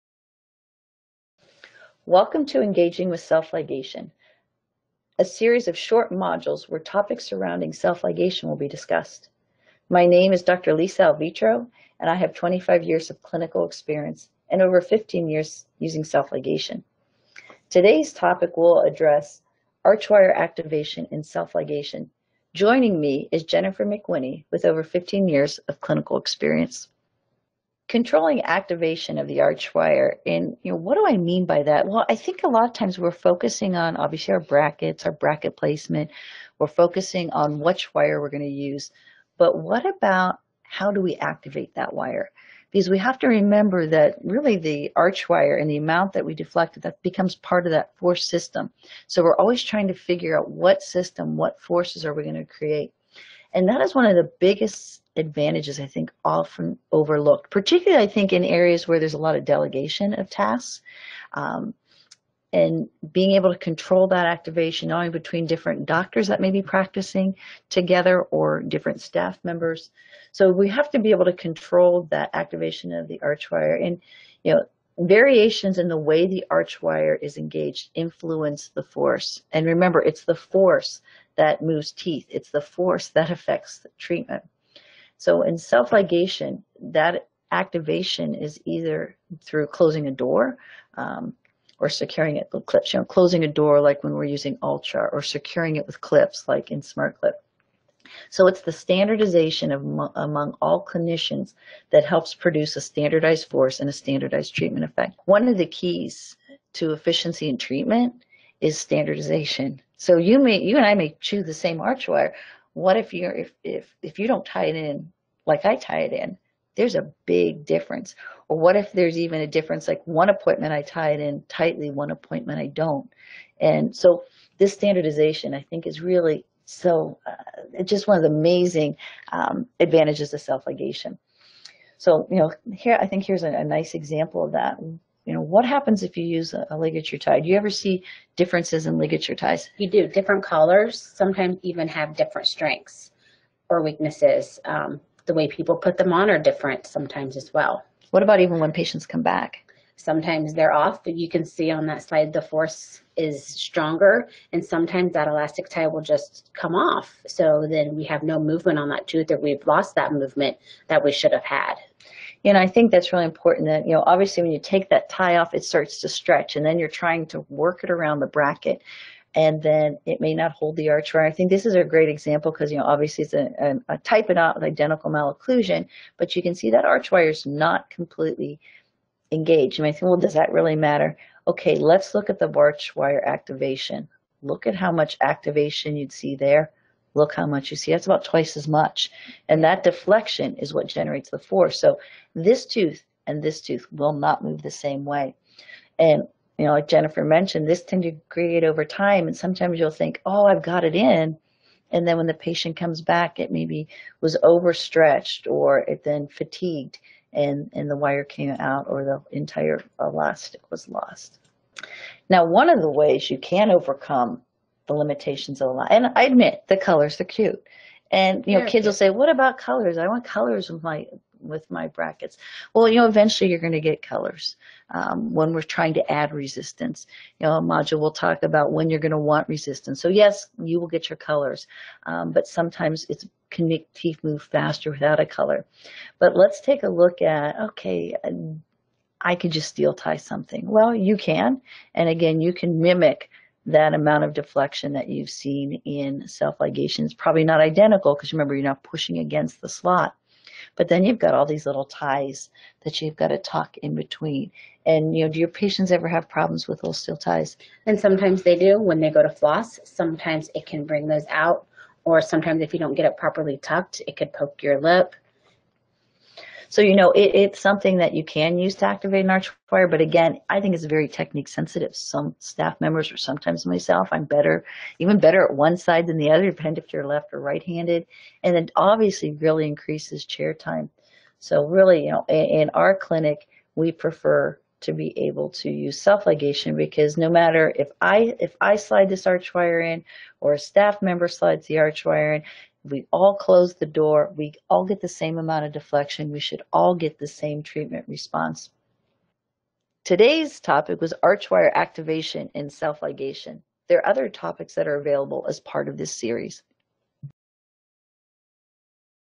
Engaging with Self-Ligation: Archwire activation in self-ligation
See how self-ligating brackets help control activation of the archwire.